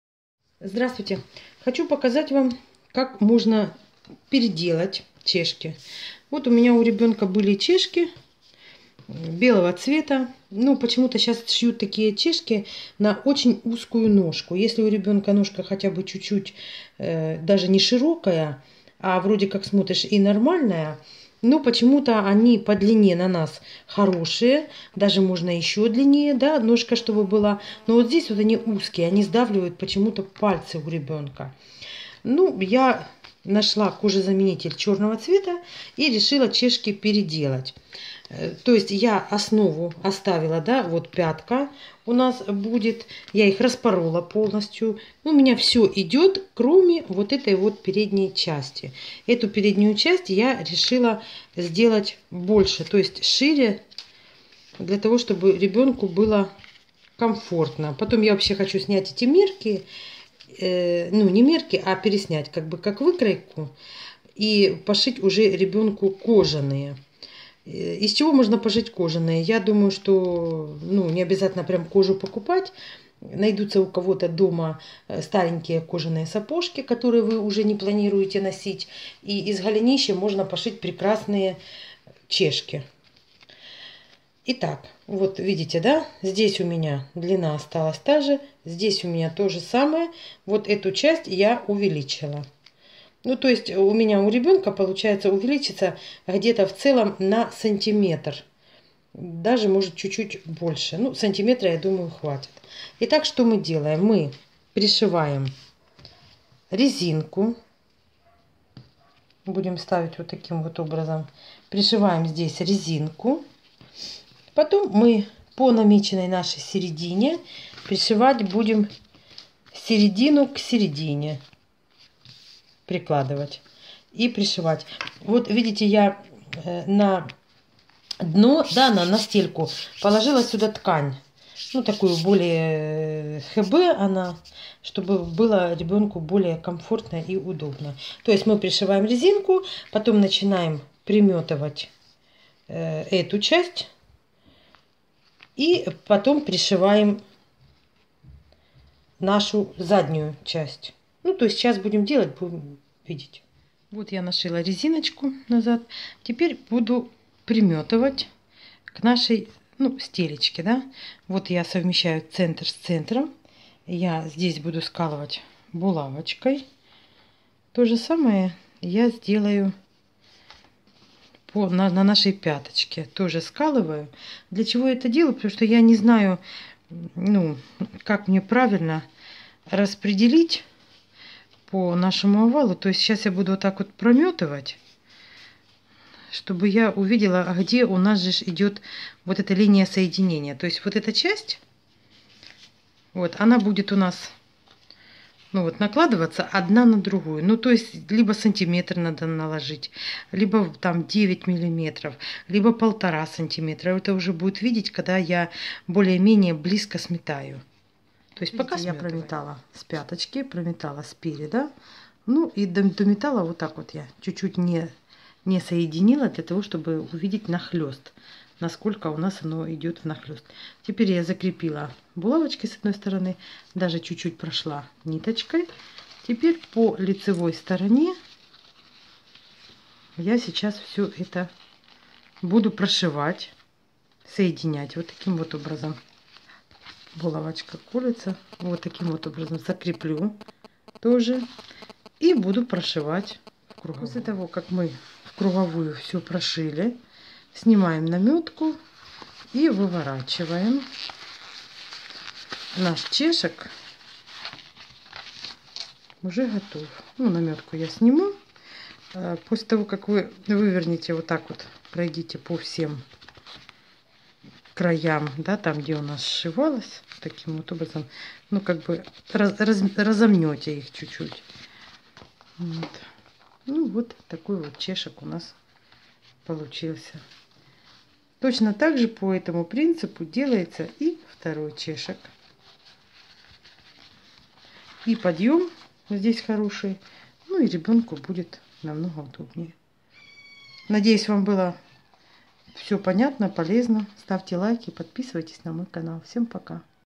Чешки детские.
Этот ролик о том как можно переделать чешки по ноге.